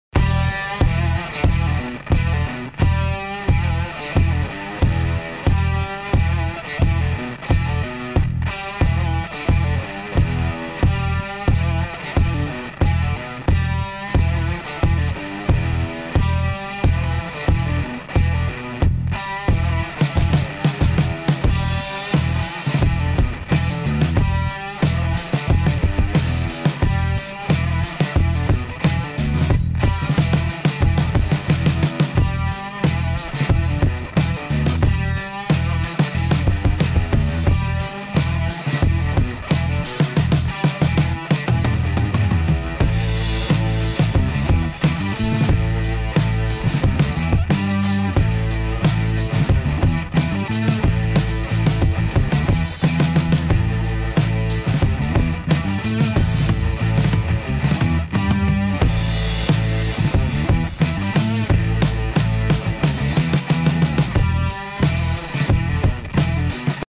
V1750 India's First RE Interceptor 650 TECH HARDY LED DRL Projector Headlight Fitted by MxsMotosport
V1750 Royal Enfield Interceptor 650 TECH HARDY LED Daymaker Projector Headlight Fitted by MxsMotosport